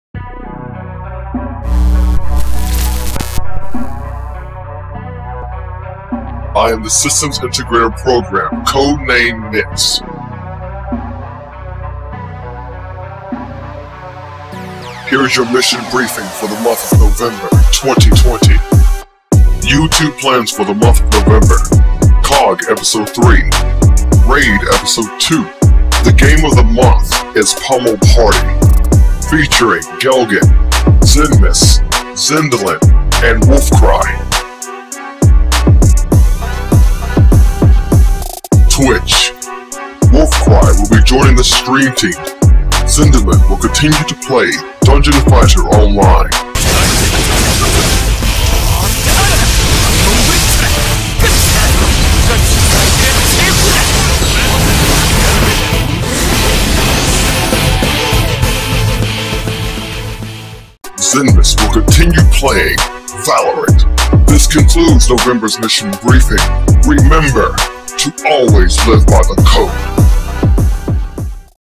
Monthly Update: November 2020
Introducing the Team Hakai's System Integration Program: NYX.

Nyx will go over the upcoming plans for Team Hakai in the month of November 2020.